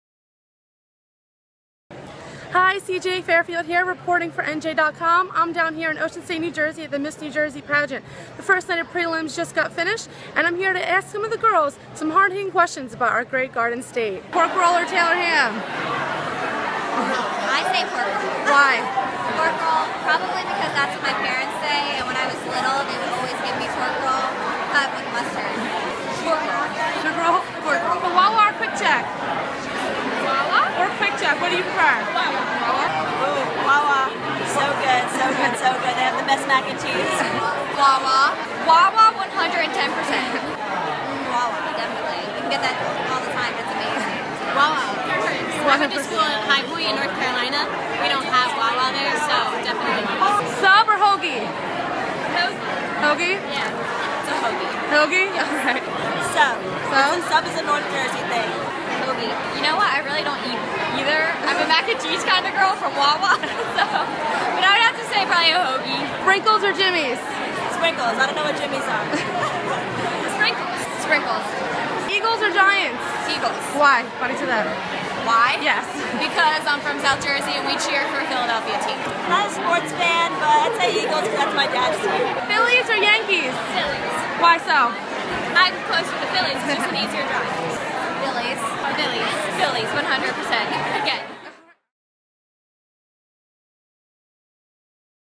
Rapid fire questions with 2016 Miss New Jersey contestants
NJ Advance Media reporter CJ Fairfield asks Jersey centric questions to the Miss New Jersey contestants after the first night of preliminaries in Ocean City. 6/16/16 (Video by CJ Fairfield | NJ Advance Media for NJ.com)